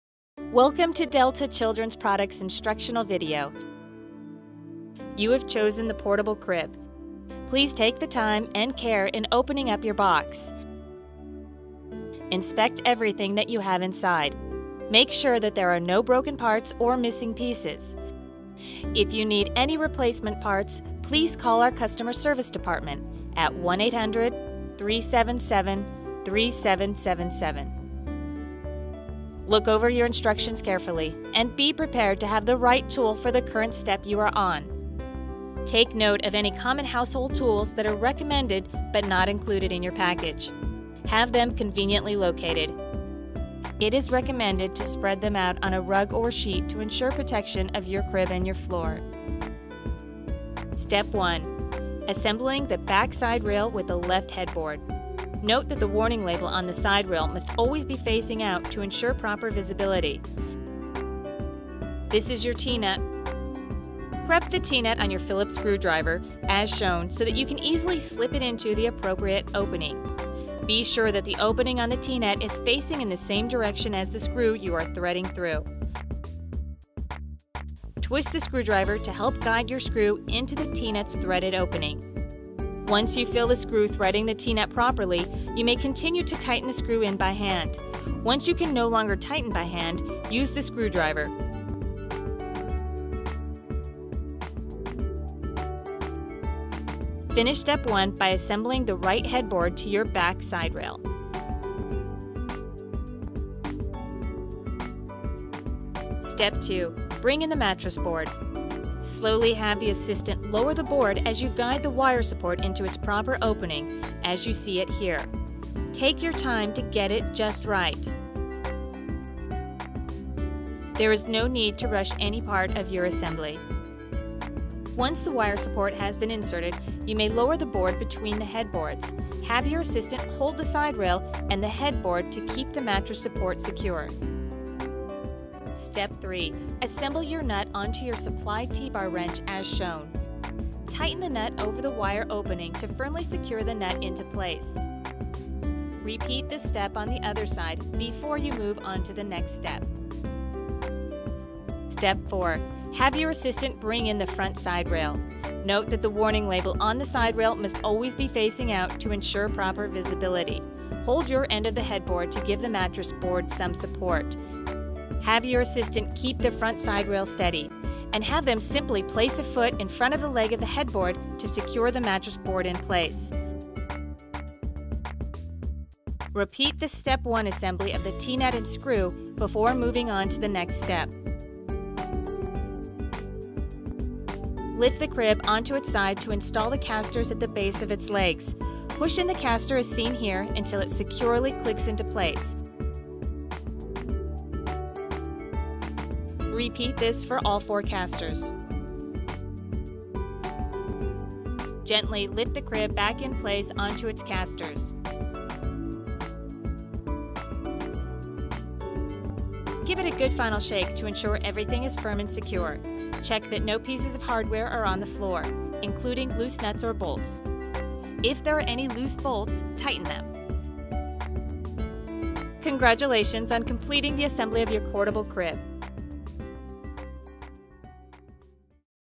Delta Portable Crib Assembly Instructions
A step by step instructional video on how to assemble a Delta Portable Crib. Thank you for choosing Delta Children's Products.